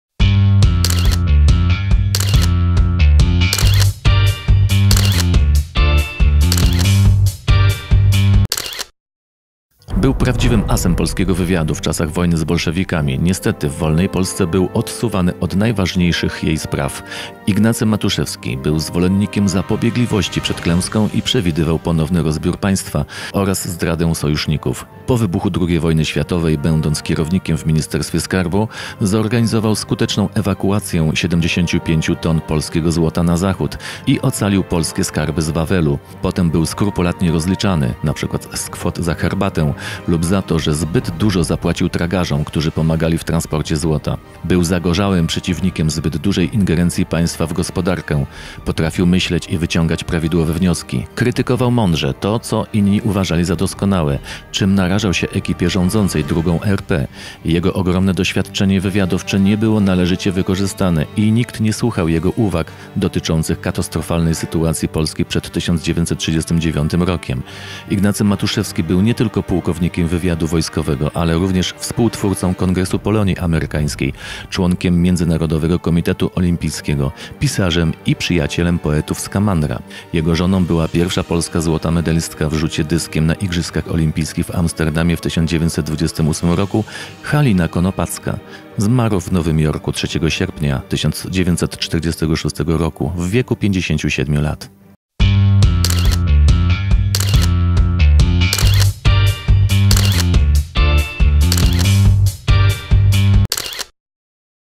Krótkie historie - Ignacy Matuszewski Niedoceniany As Wywiadu
Był prawdziwym asem polskiego wywiadu w czasach wojny z bolszewikami. Niestety, w wolnej Polsce był odsuwany od najważniejszych jej spraw. Ignacy Matuszewski. Był zwolennikiem zapobiegliwości przed klęską i przewidywał ponowny rozbiór Państwa i zdradę sojuszników. Po wybuchu II wojny światowej będąc kierownikiem w ministerstwie skarbu, zorganizował skuteczną ewakuację 75 ton polskiego złota na Zachód i ocalił polskie skarby z Wawelu. Potem był skrupulatnie rozliczany np. z kwot za herbatę lub za to, że zbyt dużo zapłacił tragarzom, którzy pomagali w transporcie złota.  Był zagorzałym przeciwnikiem zbyt dużej ingerencji państwa w gospodarkę. Potrafił myśleć i wyciągać prawidłowe wnioski. Krytykował mądrze to, co inni uważali za doskonałe, czym narażał się ekipie rządzącej II RP. Jego ogromne doświadczenie wywiadowcze nie było należycie wykorzystane i nikt nie słuchał jego uwag dotyczących katastrofalnej sytuacji Polski przed 1939 rokiem. Ignacy Matuszewski był nie tylko pułkownikiem wywiadu wojskowego, ale również współtwórcą Kongresu Polonii Amerykańskiej, członkiem MKO, pisarzem i przyjacielem poetów skamandra.
Jego żoną była pierwsza polska złota medalistka w rzucie dyskiem na igrzyskach olimpijskich w Amsterdamie w 1928r Halina Konopacka. Zmarł w Nowym Jorku 3 sierpnia 1946r w wieku 57 lat.